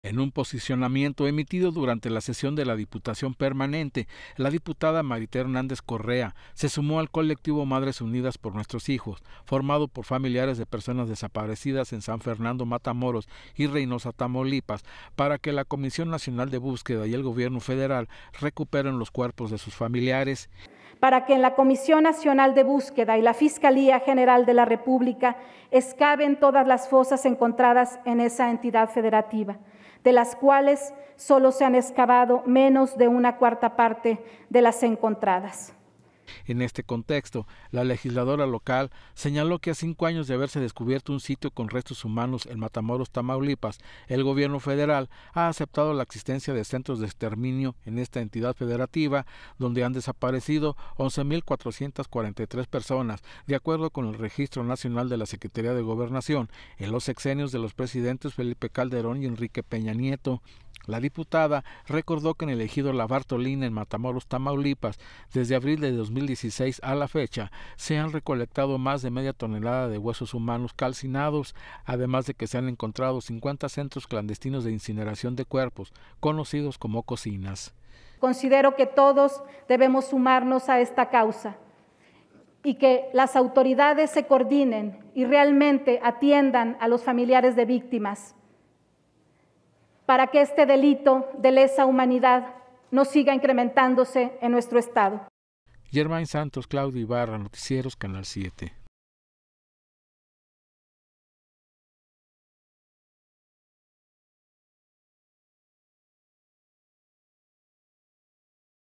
Hernández Correa se suma al colectivo “Madres Unidas por Nuestros Hijos”.
• La organización se dedica a la búsqueda y localización de personas desaparecidas.

SLP.- En un posicionamiento emitido durante la sesión de la Diputación Permanente la diputada Marite Hernández Correa, se sumó al colectivo Madres Unidas por Nuestros Hijos, formado por familiares de personas desaparecidas en San Fernando, Matamoros y Reynosa, Tamaulipas, para que la Comisión Nacional de Búsqueda y el gobierno federal recuperen los cuerpos de sus familiares.

En este contexto la legisladora local señaló que a cinco años de haberse descubierto un sitio con restos humanos en Matamoros, Tamaulipas, el gobierno federal ha aceptado la existencia de centros de exterminio en esta entidad federativa, donde han desaparecido 11 mil 443 personas de acuerdo con el registro nacional de la Secretaría de Gobernación, en los sexenios de los presidentes Felipe Calderón y Enrique Peña Nieto.

La diputada recordó que en el ejido La Bartolina en Matamoros Tamaulipas, desde abril de 2016 a la fecha se han recolectado más de media tonelada de huesos humanos calcinados, además de que se han encontrado 50 centros clandestinos de incineración de cuerpos, conocidos como “cocinas”.